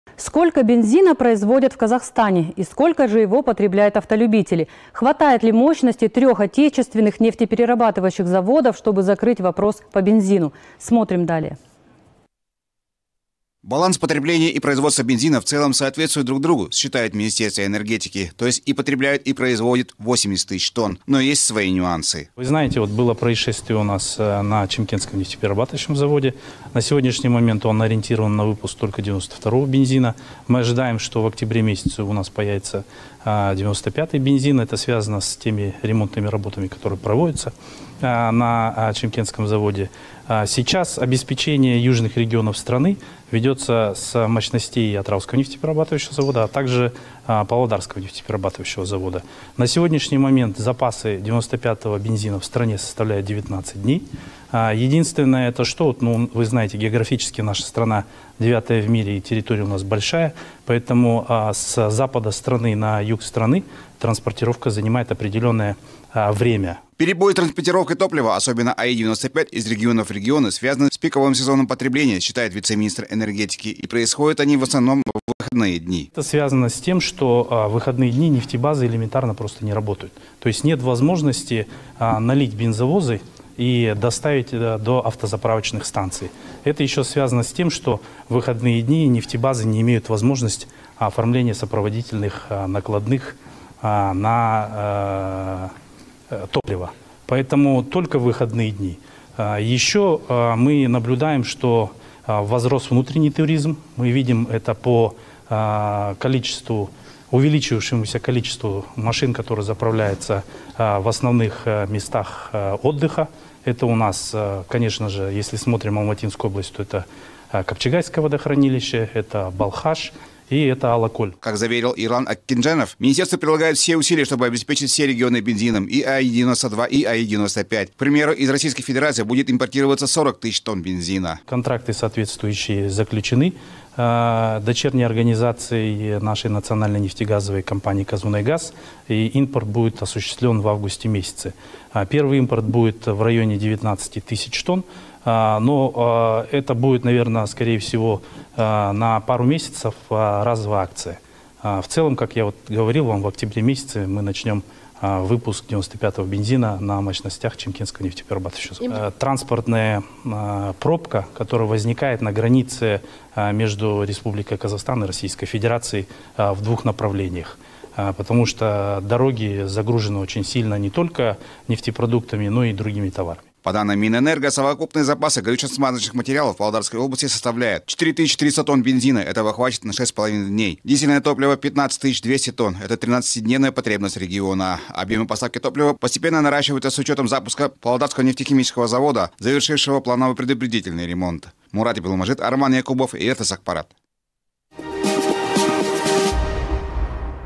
Ситуация с бензином
Сколько бензина производят в Казахстане? И сколько его же потребляют автолюбители? Хватает ли мощностей трёх отечественных нефтеперерабатывающих заводов, чтобы закрыть вопрос по бензину?